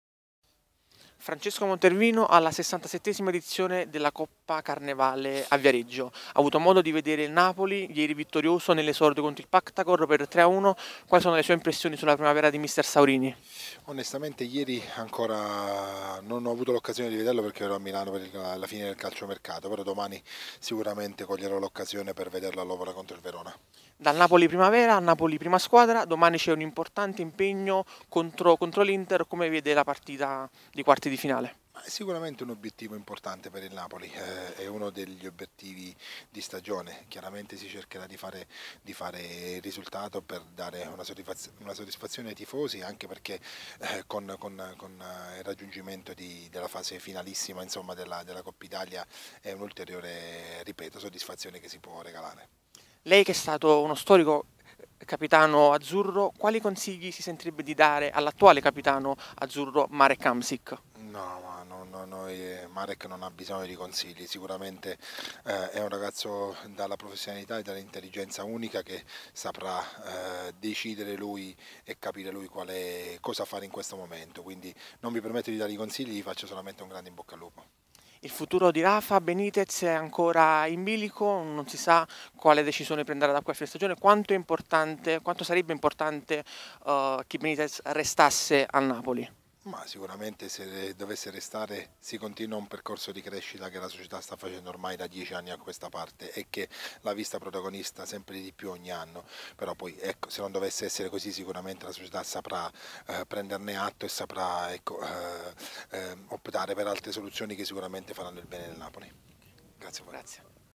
Montervino a SN: "La Coppa Italia può dare tante soddisfazioni. Hamsik? Esempio di professionalità"
Francesco Montervino, ex giocatore del Napoli tra il 2004 e il 2009, presente al Torneo di Viareggio per visionare giovani talenti per il suo Taranto, è intervenuto in esclusiva ai microfoni di SpazioNapoli.it